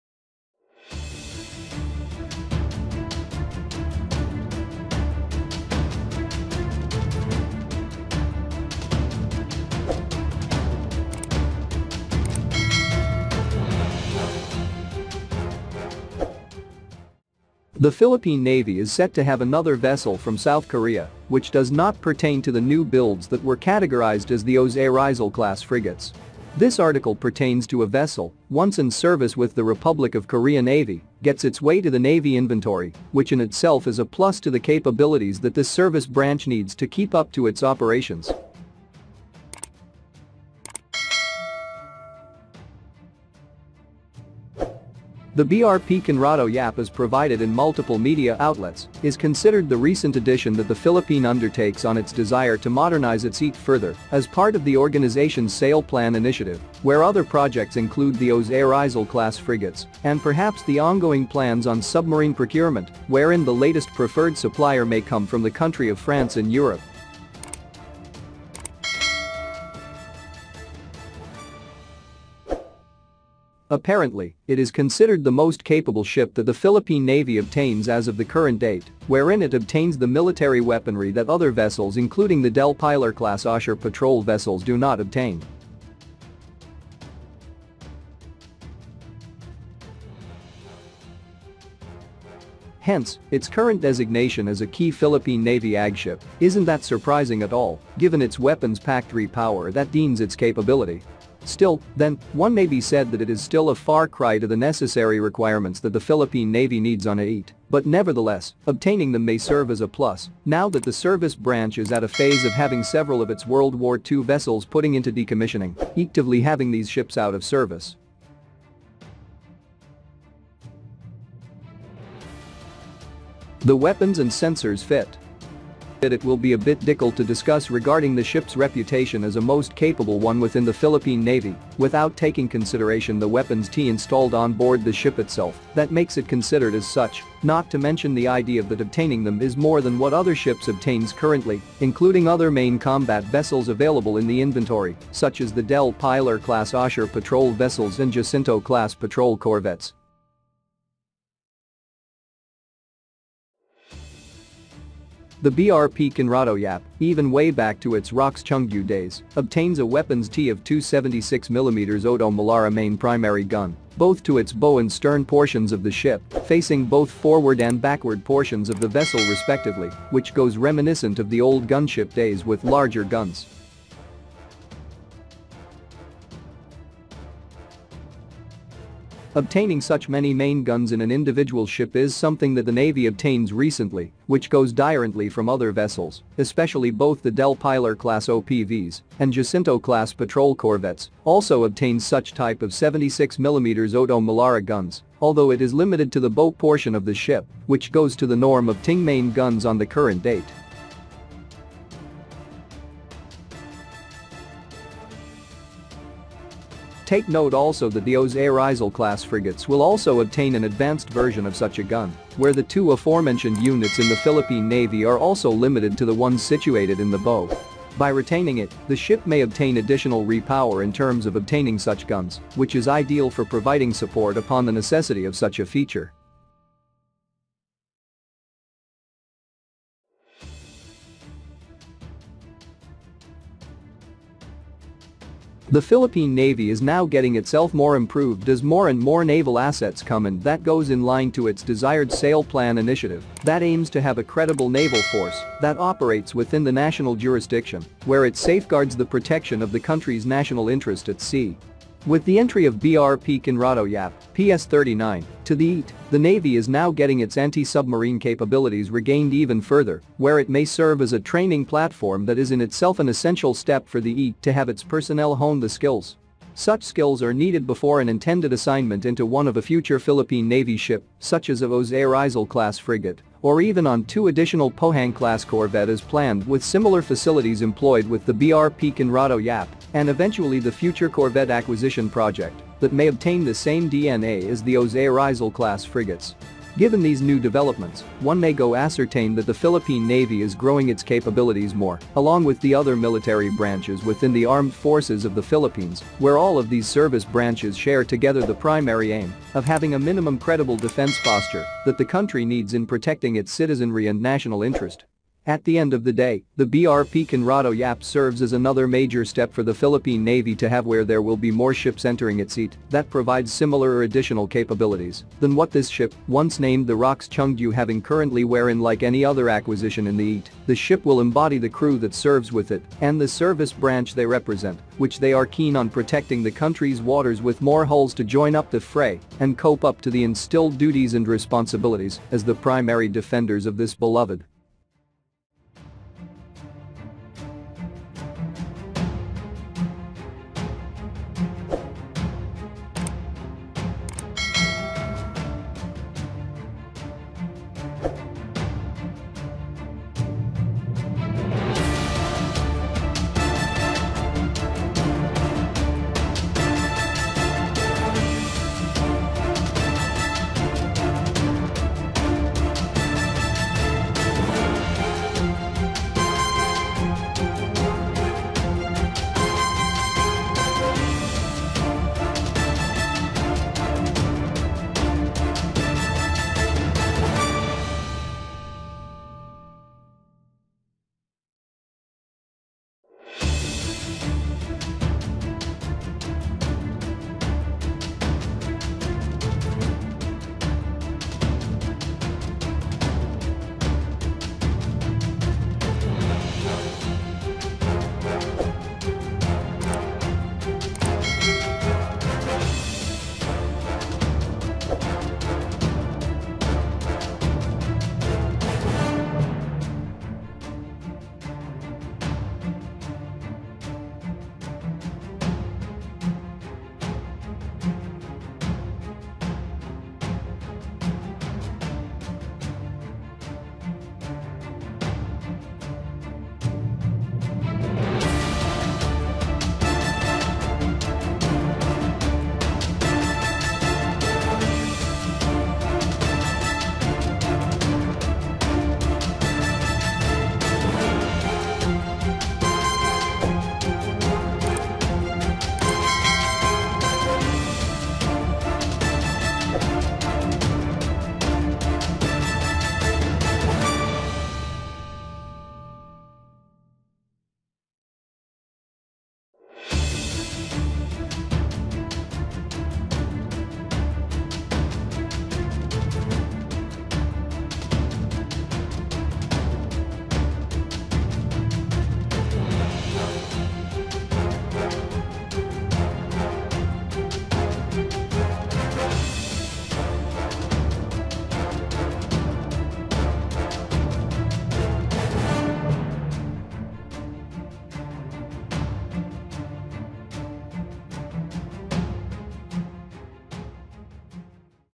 Great! The Philippine Navy is set to Have Several Vessels From South Korea
The Philippine Navy is set to have another vessel from South Korea which does not pertain to the new builds that were categorized as the Jose Rizal-class Frigates.

The BRP Conrado Yap as provided in multiple media outlets is considered the recent addition that
the Philippine undertakes on its desire to modernize its eet further as part of the organization's
Sail Plan initiative where other projects include the Jose Rizal-class Frigates and perhaps the ongoing plans on submarine procurement wherein the latest preferred supplier may come from the
country of France in Europe.